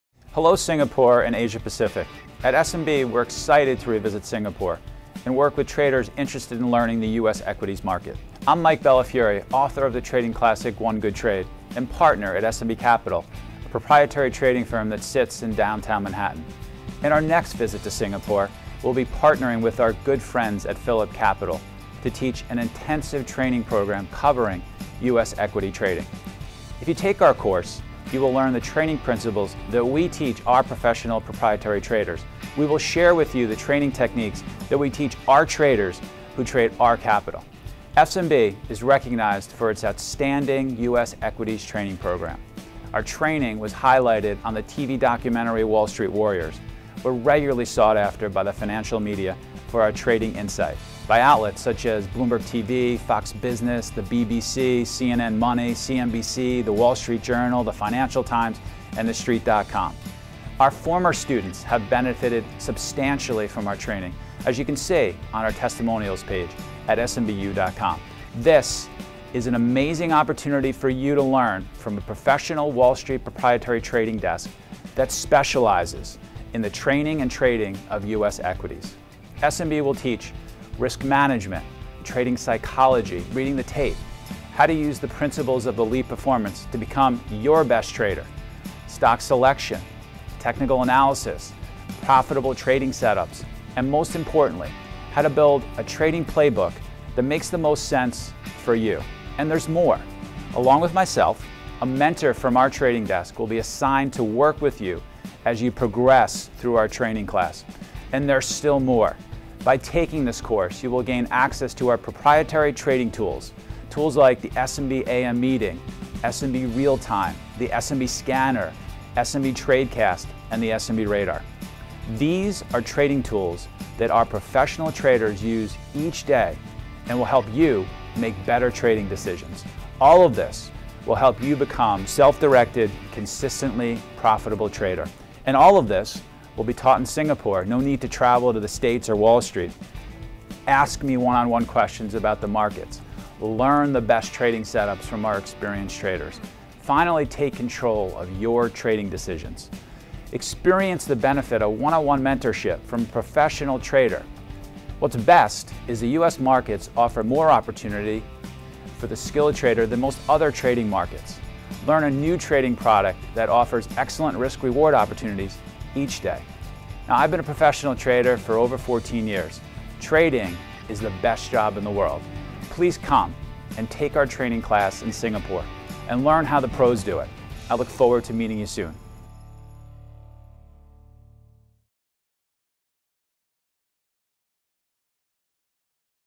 Wall Street Weekend with Mike Bellafoire Seminar (24 Aug 2012 in Singapore)
Mr Mike Bellafiore is the author of "One Good Trade: Inside the Highly Competitive World of Proprietary Trading" and co-founder of SMB Capital, a proprietary trading firm in New York City. SMB has been recognised for offering an outstanding intraday equities training program. Mike will share the lessons the market has taught him over the past 15 years, wrapped inside trading stories and his own trades.

SMB Capital regularly appears on CNBC's Fast Money while its training programme has been highlighted on the TV documentary "Wall Street Warriors". Mike's trading techniques were noted in "The Daily Coach: 101 Lessons for Becoming Your Own Trading Psychologist", written by Dr. Steenbarger. Mr Bellafiore lectures internationally and was a guest speaker at the 2009, 2010 and 2011 International Traders Expo in New York City. Mr Bellafiore has written numerous articles for SFO Magazine, most recently about how to turn trading knowledge into skill.